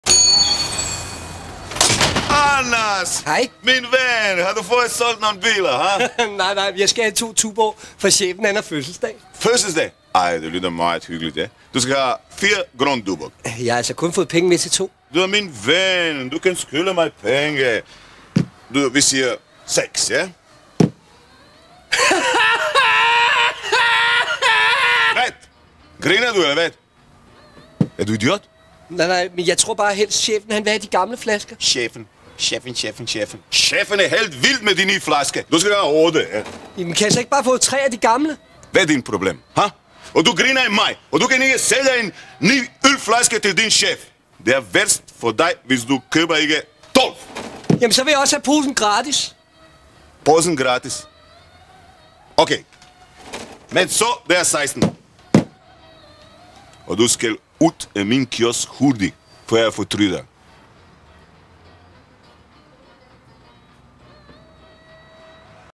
Reklame for Tuborg Plastik øl, "Duborg"
Med Zlatko Buric og Søren Fauli. Sendt år 2000.